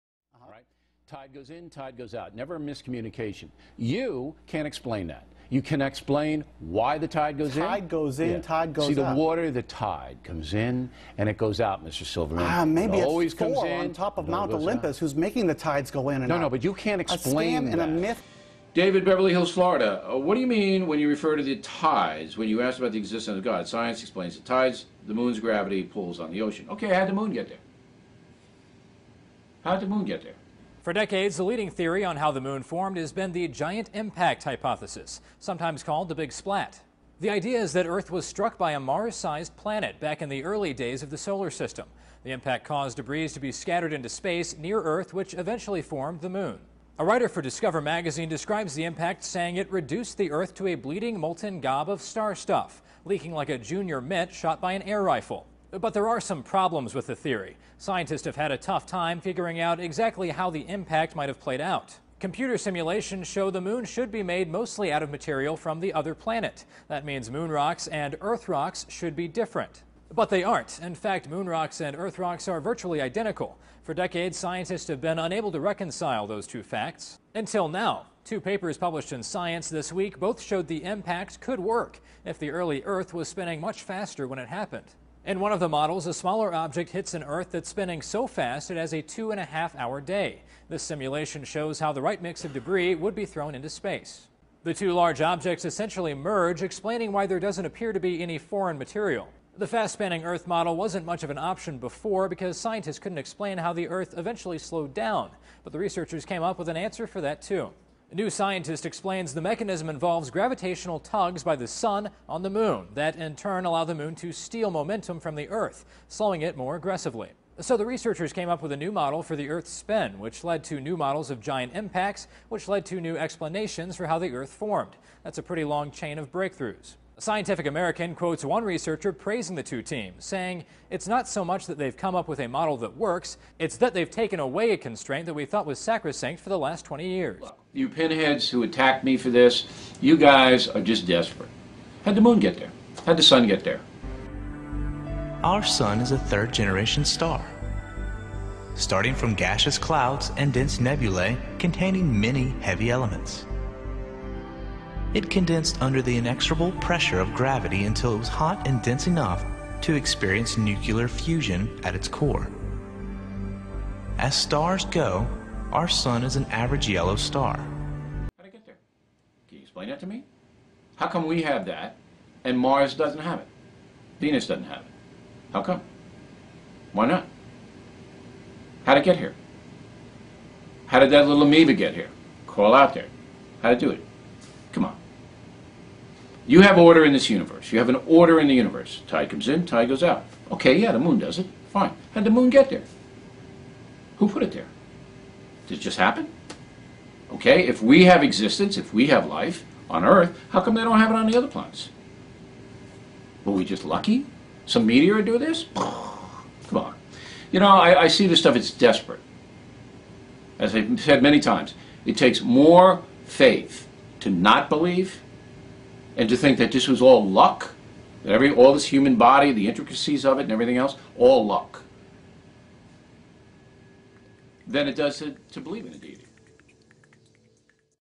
Tide Goes In Tide Goes Out Explained By Science; Bill O'Reilly is an idiot!
Bill O'Reilly offers his best argument for the existence of God, but ends up proving that he is either too stupid or ignorant of basic Science, or that he expects you to be. Or both.